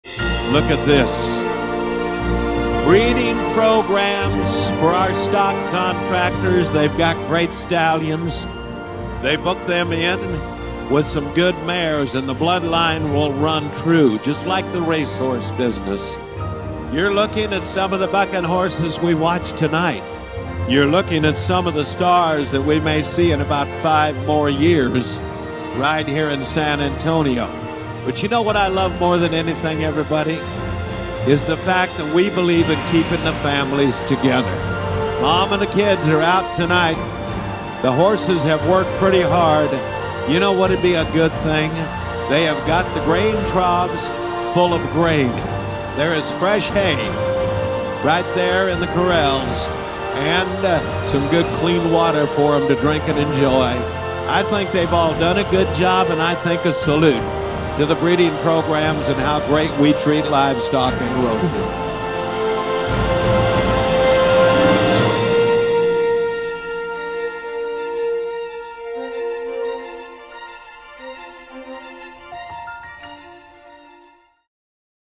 Mares and Foals in San Antonio
"We keep the families together". The bucking mares and their foals at the San Antonio Stock Show and Rodeo might be one of the most beautiful things you will see in any arena anywhere.